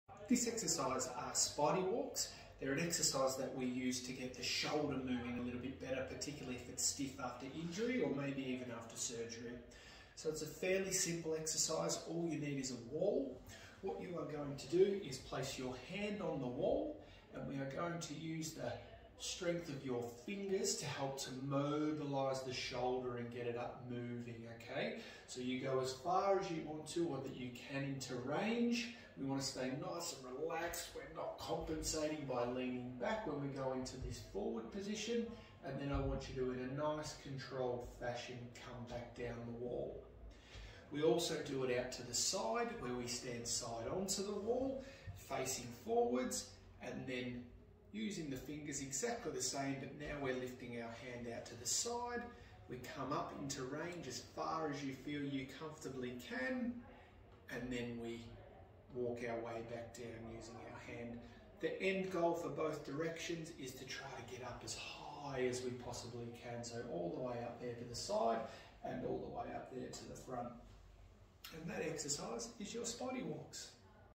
Spidey Walks
An exercise to help get stiff and weak shoulders moving